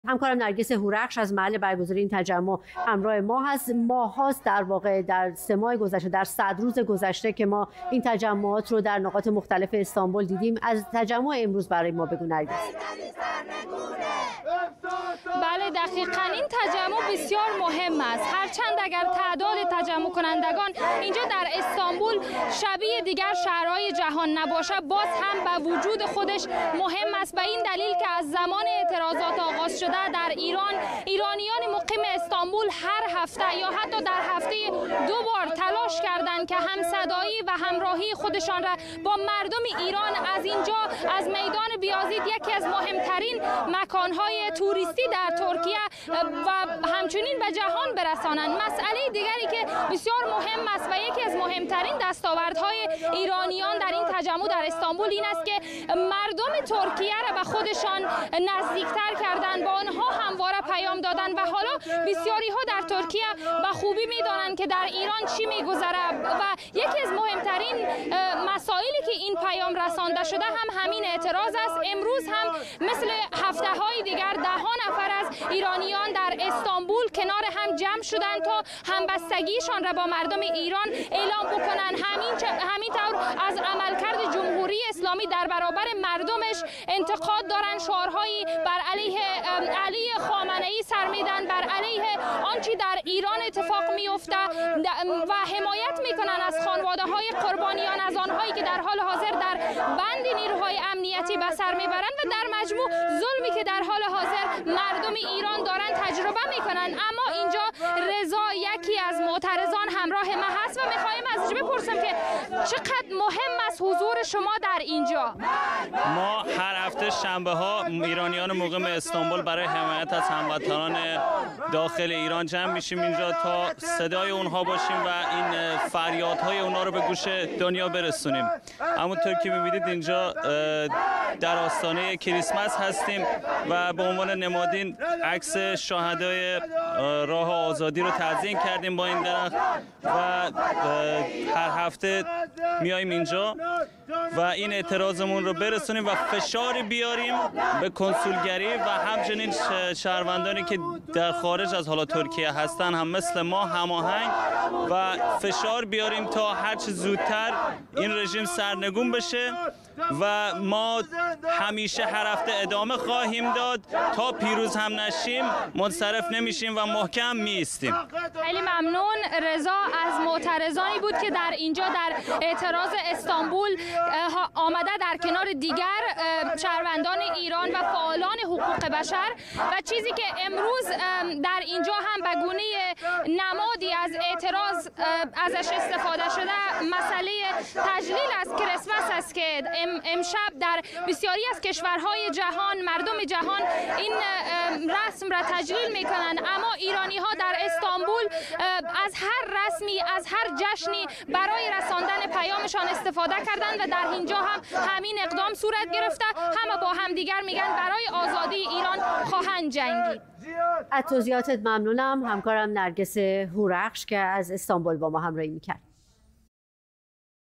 تجمع ایرانیان مقیم ترکیه در استانبول، در حمایت از خیزش انقلابی ایرانیان
تجمع ایرانیان مقیم ترکیه در استانبول، در حمایت از خیزش انقلابی ایرانیان علیه جمهوری اسلامی
گزارش نرگس هورخش، ایران‌اینترنشنال، استانبول